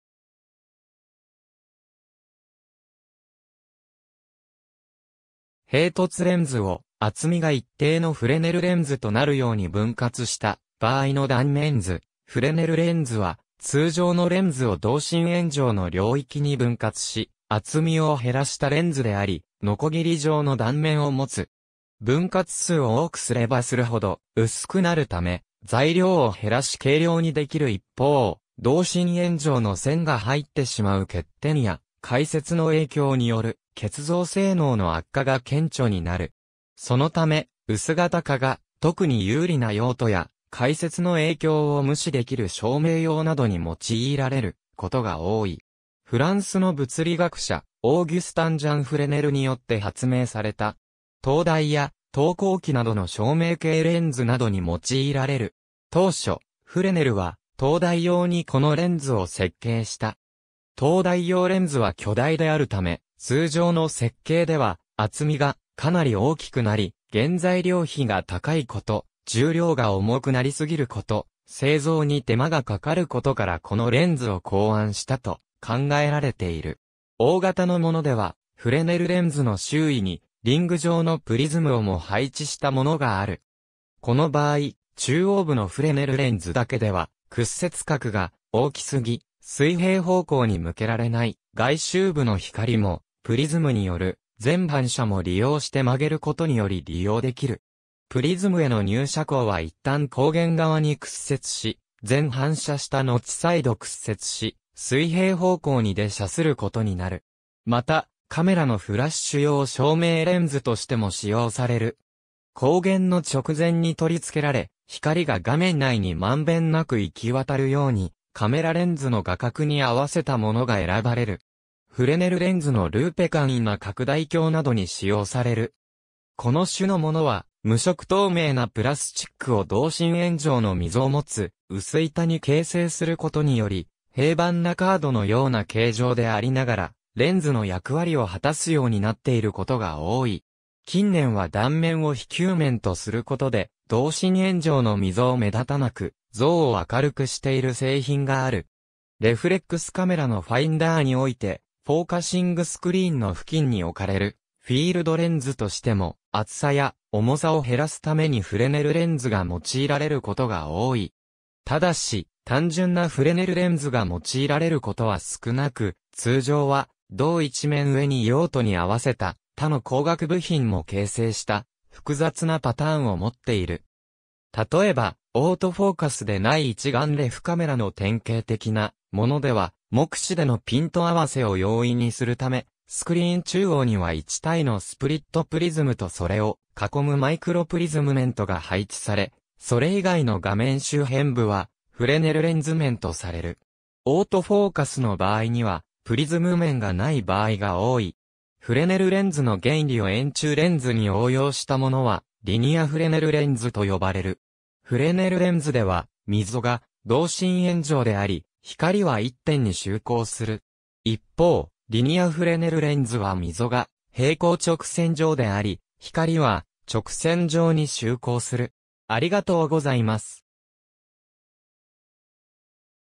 フレネルレンズ
平凸レンズ（上）を厚みが一定のフレネルレンズ（下）となるように分割した場合の断面図 フレネルレンズ（英: Fresnel lens [freɪˈnɛl lɛnz, ˈfrɛznəl lɛnz]）は、通常のレンズを同心円状の領域に分割し厚みを減らしたレンズであり、のこぎり状の断面を持つ。
分割数を多くすればするほど薄くなるため、材料を減らし軽量にできる一方、同心円状の線が入ってしまう欠点や、回折の影響による結像性能の悪化が顕著になる。
そのため、薄型化が特に有利な用途や、回折の影響を無視できる照明用などに用いられることが多い。
フランスの物理学者オーギュスタン・ジャン・フレネルによって発明された。
灯台や投光器などの照明系レンズなどに用いられる。
当初、フレネルは、灯台用にこのレンズを設計した。
灯台用レンズは巨大であるため、通常の設計では厚みがかなり大きくなり原材料費が高いこと、重量が重くなりすぎること、製造に手間がかかることからこのレンズを考案したと考えられている。
大型の物では、フレネルレンズの周囲にリング状のプリズムをも配置した物がある。
この場合、中央部のフレネルレンズだけでは屈折角が大きすぎ水平方向に向けられない外周部の光も、プリズムによる全反射も利用して曲げることにより利用できる。
プリズムへの入射光は一旦光源側に屈折し、全反射したのち再度屈折し水平方向に出射することになる（プリズムの三角の向きがフレネルレンズと逆であることに注意）。
また、カメラのフラッシュ用照明レンズとしても使用される。
光源（通常はキセノンフラッシュランプ）の直前に取り付けられ、光が画面内にまんべんなく行き渡るようにカメラレンズの画角に合わせたものが選ばれる。
フレネルレンズのルーペ 簡易な拡大鏡などに使用される。
この種のものは無色透明なプラスチック（アクリル樹脂、ポリスチレン、ポリカーボネートなど）を同心円状の溝を持つ薄板に形成することにより、平板なカードの様な形状でありながらレンズの役割を果たすようになっていることが多い。
近年は断面を非球面とすることで、同心円状の溝を目立たなく、像を明るくしている製品がある。
レフレックスカメラ（一眼レフや二眼レフ）のファインダーにおいてフォーカシングスクリーン（すりガラス面など）の付近に置かれるフィールドレンズとしても、厚さや重さを減らすためにフレネルレンズが用いられる事が多い。
ただし、単純なフレネルレンズが用いられることは少なく、通常は同一面上に用途に合わせた他の光学部品も形成した複雑なパターンを持っている。
例えば、オートフォーカスでない一眼レフカメラの典型的なものでは、目視でのピント合わせを容易にするため、スクリーン中央には一対のスプリットプリズムとそれを囲むマイクロプリズム面とが配置され、それ以外の画面周辺部はフレネルレンズ面とされる。
オートフォーカスの場合にはプリズム面が無い場合が多い。
フレネルレンズの原理を円柱レンズに応用したものはリニアフレネルレンズと呼ばれる。
フレネルレンズでは溝が同心円状であり、光は一点に集光する。
一方、リニアフレネルレンズは溝が平行直線状であり、光は直線上に集光する。